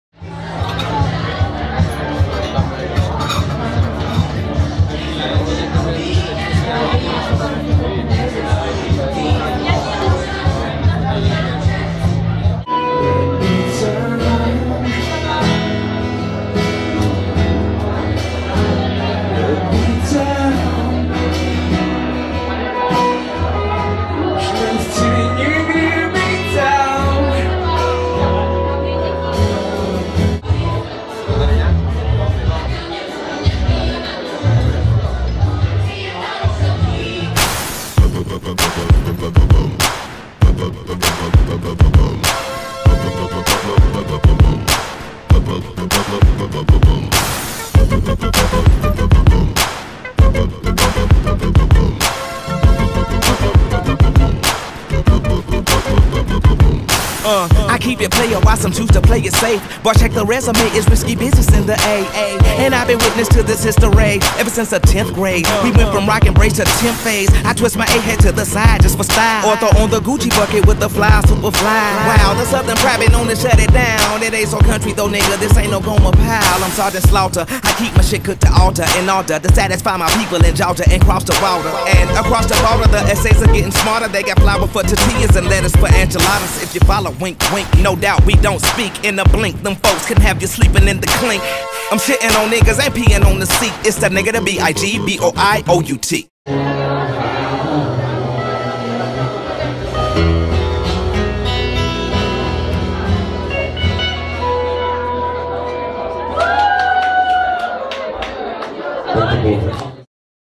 ČILINEWS: Hlasité noci ve Špindlu
Jak vypadají vyhlášené tahy zlaté mládeže ve Špindlerově Mlýně? Taky nás to zajímalo. Vyslali jsme tedy reportéra přímo do centra dění. Když se vrátil, týden nemluvil. Z jeho kamery jsme ale vydolovali tohle.